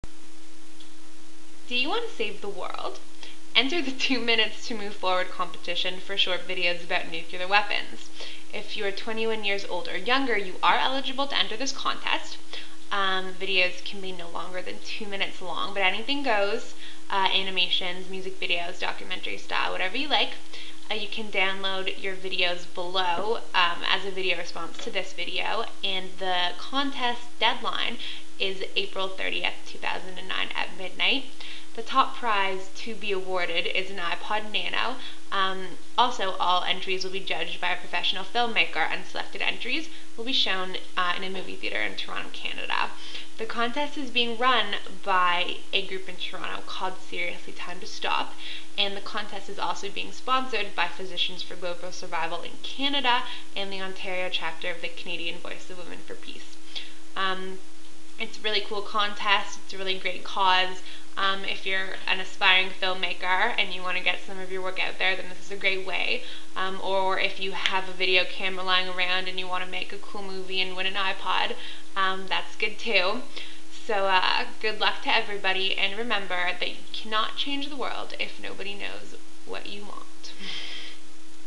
SERIOUSLY, TIME TO STOP! VIDEO COMPETITION! PRIZES!
Seriously, Time To Stop is looking for short videos (2 minutes max) about nuclear weapons. Anything goes - animations, music videos or straight ahead documentary style! Selected videos will be shown at a local cinema in Toronto and videos will be judged by professional film maker!

First prize: iPod classic
Second Prize: iPod nano

You must be under 22 to enter and must have your videos in by April 30th, 2009. FREE TO ENTER!